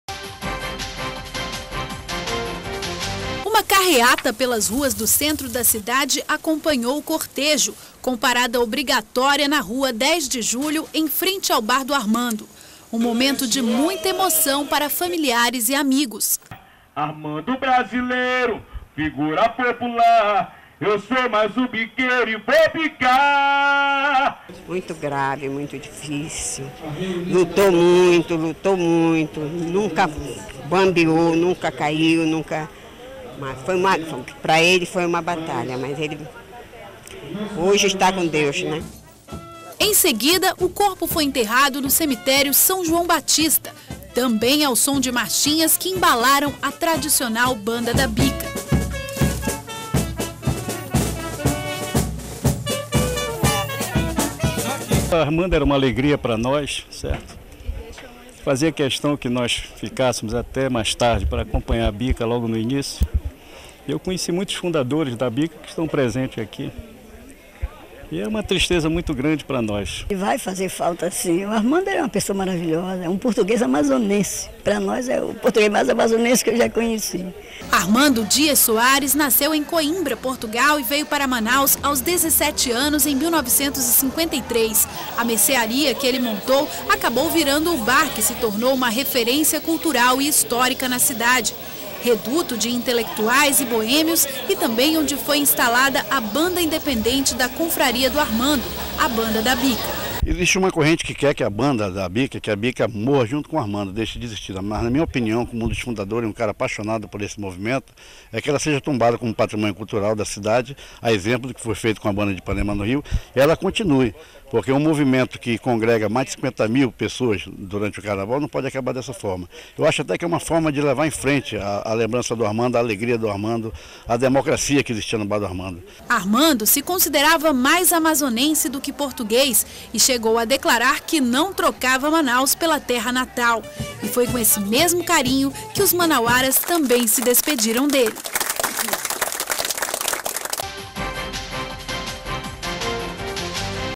Armando Soares é enterrado após homenagem, em Manaus
O português, Armando Dias Soares, dono do tradicional "Bar do Armando", foi enterrado na tarde desta quarta-feira (11) no Cemitério São João Batista, em Manaus.  Com presença de pessoas ilustres, Armando foi homenageado pela última vez em frente ao seu bar pela Escola de Samba Reino Unido da Liberdade.